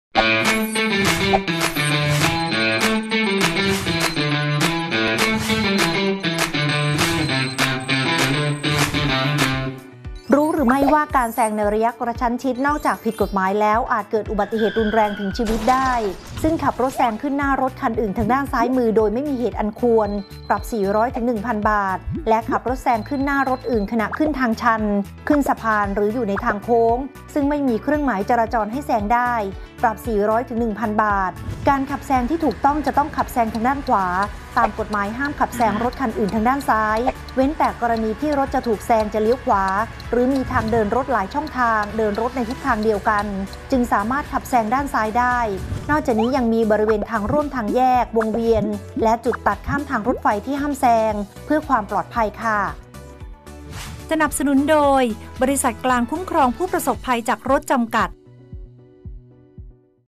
ตอนที่ 470 ตอน การแซงในระยะกระชั้นชิด ผิดกฎหมาย อาจเกิดอุบัติเหตุรุนแรงถึงชีวิต
บริษัท กลางคุ้มครองผู้ประสบภัยจากรถ จำกัด